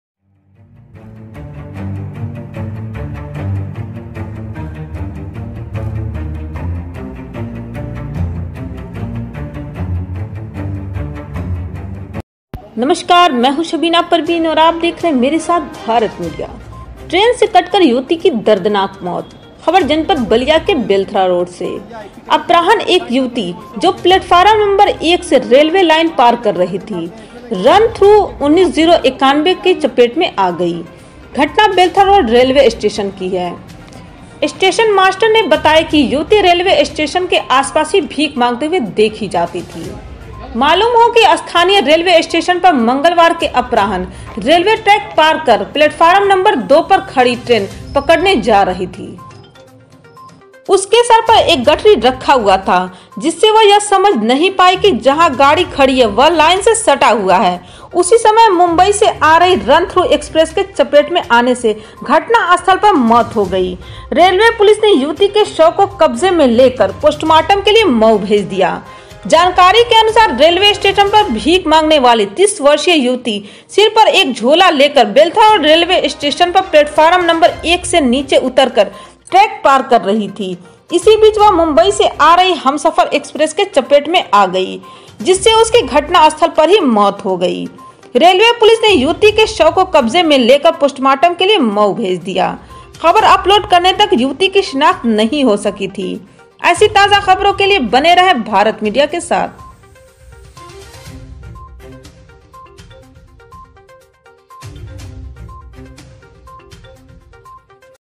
Belthara Road || ट्रेन से कटकर युवती की दर्दनाक मौत
ट्रेन से कटकर युवती की दर्दनाक मौत